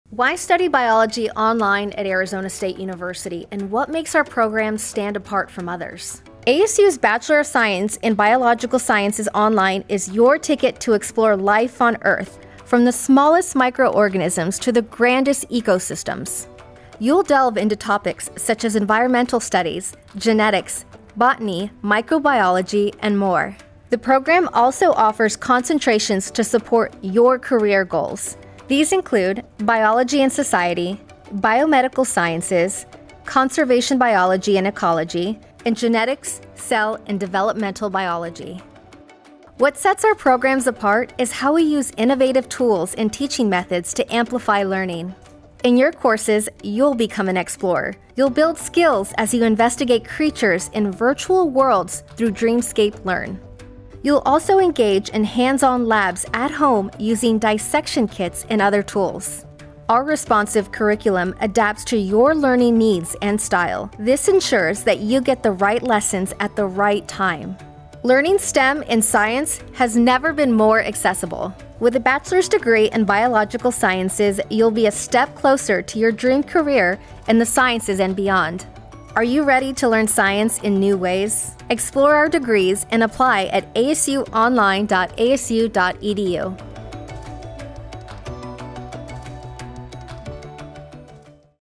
Discover your path in biological sciences at ASU Online
Are you interested in earning an undergraduate degree that explores topics such as environmental studies, genetics, botany, microbiology and more? In this video, you’ll learn about the different biological sciences concentrations offered in this specialized program and discover which best suits your goals.

Learn more about the differences between ASU Online’s biological sciences concentrations